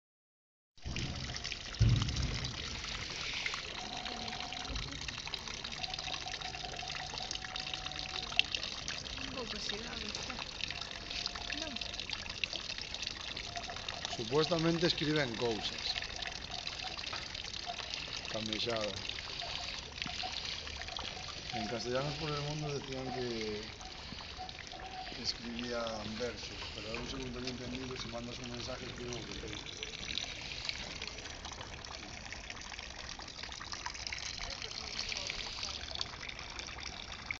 Museo Kafka Praga Feb 2013
A fonte é obra do artista local David Černý . Dous homes mexan nunha representación do mapa da República Checa. Cos movementos din que o que debuxan son frases de praguenses famosos. Din tamén que se pode interactuar coa fonte co envío de textos en  SMS que acaban representando coas mexadas. Con Tera, @NeereaBB e @AnxinhoIA